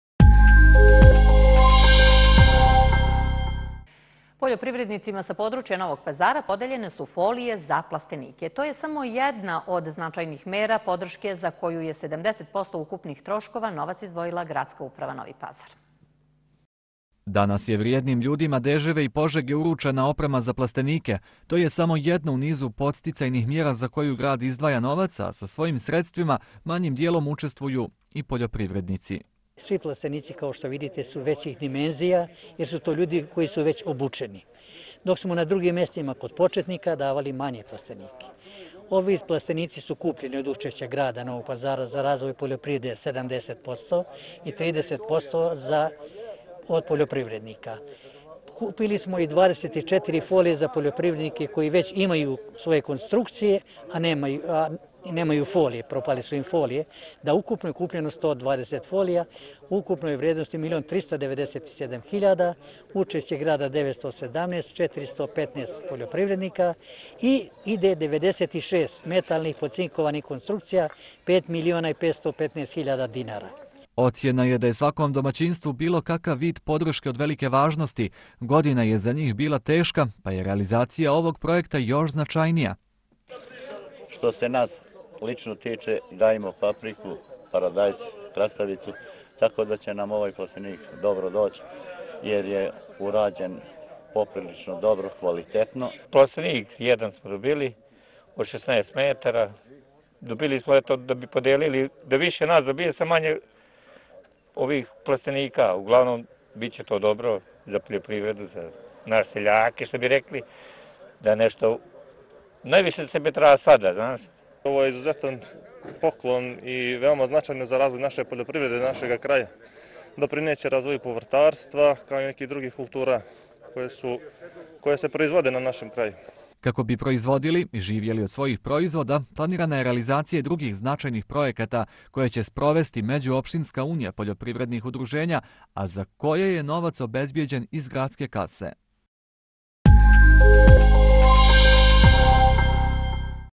Novopazarskim poljoprivrednicima podeljene folije za plastenike
Poljoprivrednicima sa područja Novog Pazara podeljene su folije za plastenike. To je samo jedna od značajnih mera podrške za koju je 70% ukupnih troškova novac izdvojila Gradska uprava Novi Pazar.